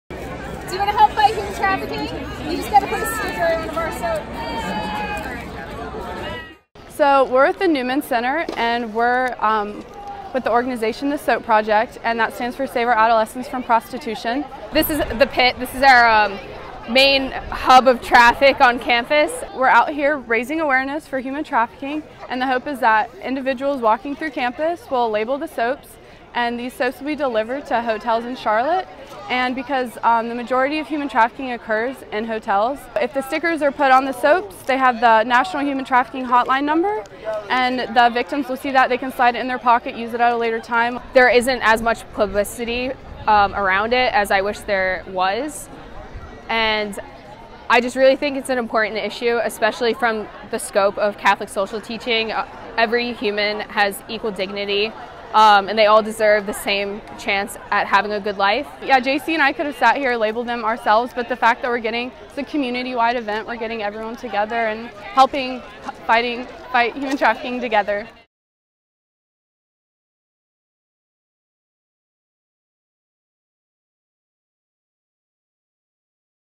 Local university students fight global problem
It’s lunchtime at “the pit,” a well-known hub on the campus of UNC Chapel Hill. Students Kara Cody and Jaycee Reilly, who belong to the Newman Catholic Community, are working a table. Instead of promoting a prize wheel or registration form, though, they are asking students to put stickers on bars of soap … and help fight human trafficking.